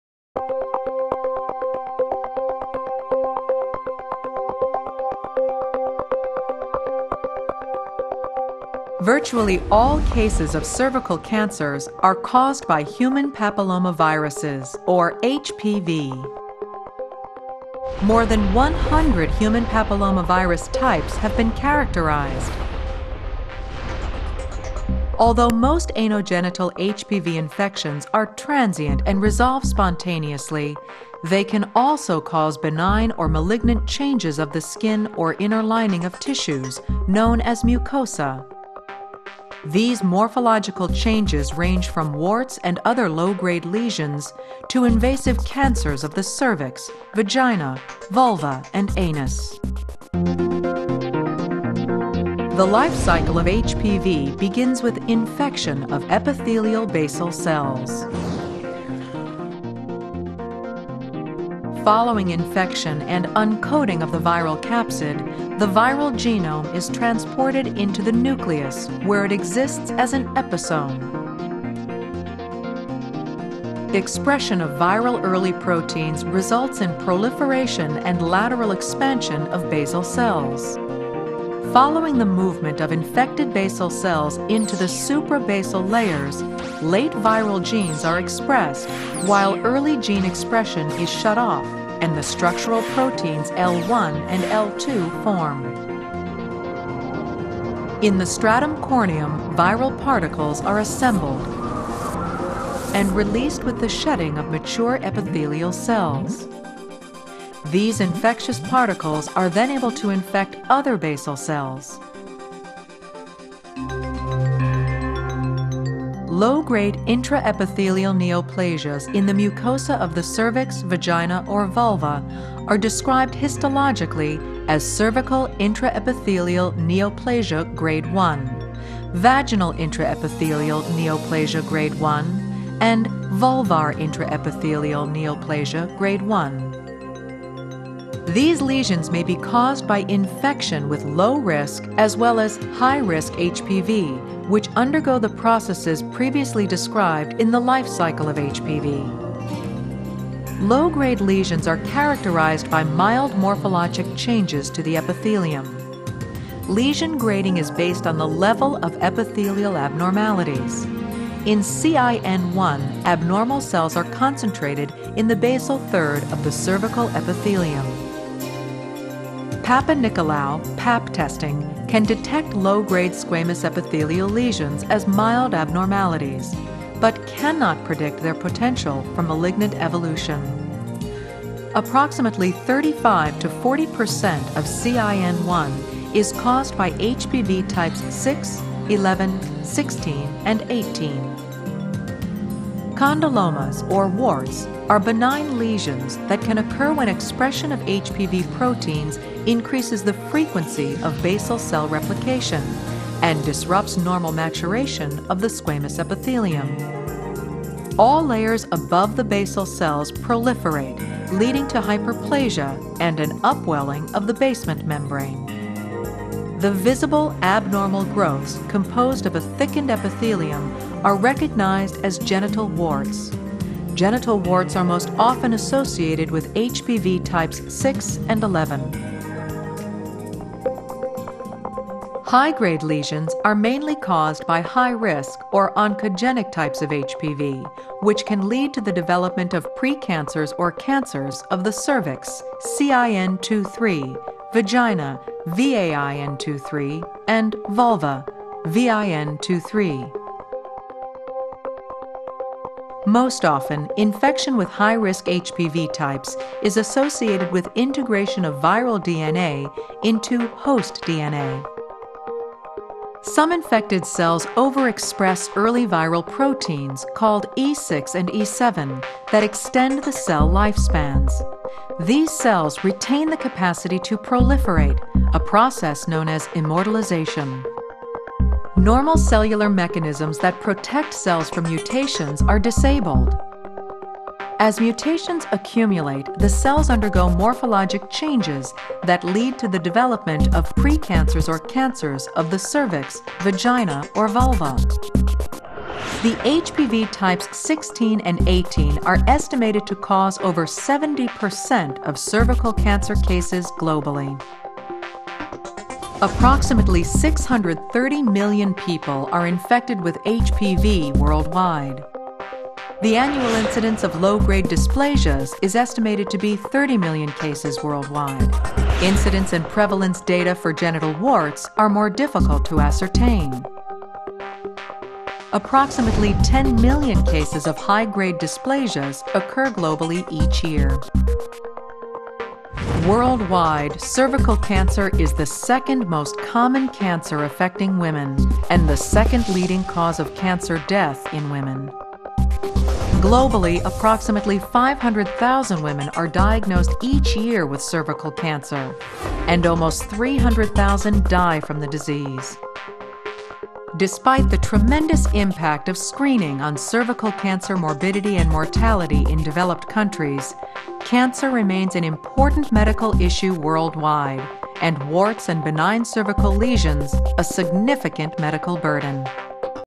Genital Warts (HPV - Movie)
Genital warts and cervical cancer can be caused by Human Papilloma Virus полово възпитание